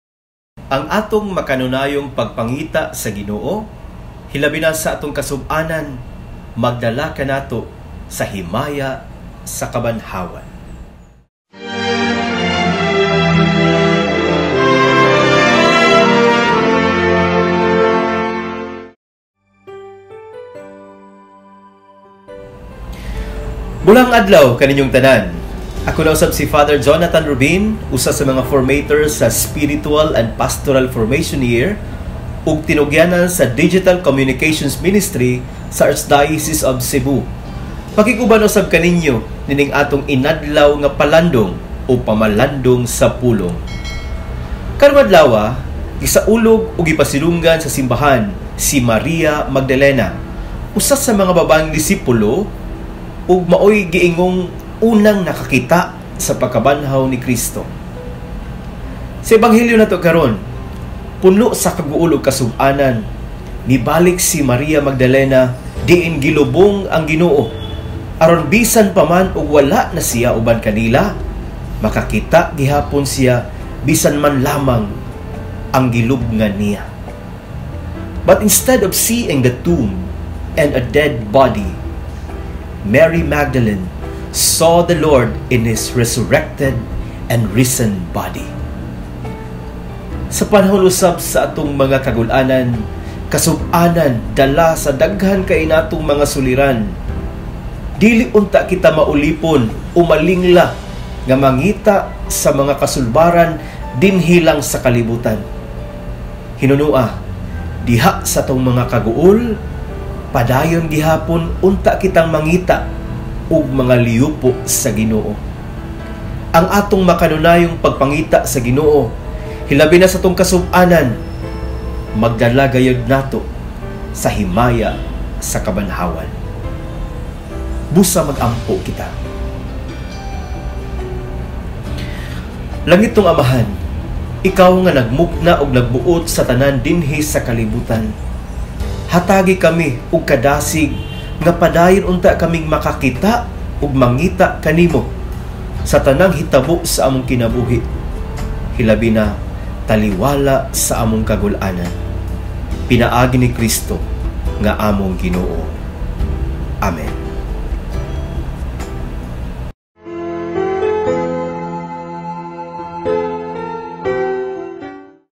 PALANDONG Season II | The Daily Gospel Reflection | Thursday - 07/22/2021
PALANDONG Season II | The Daily Gospel Reflection

Feast of Saint Mary Magdalene, Apostle of the Apostles
16th Week in the Ordinary Time
"Ang atong makanunayong pagpangita sa Ginoo, Hilabina sa atong kasub-anan, Magdala kanato sa himaya sa kabanhawan."
A Gospel excerpt from John 20:1-2,11-18

By Rev. Fr. Jonathan Rubin,  M.A.P.M., SThL/M.A.
Formator, Spiritual & Pastoral Formation Year
Director, Cebu Archdiocesan Digital Communications Ministry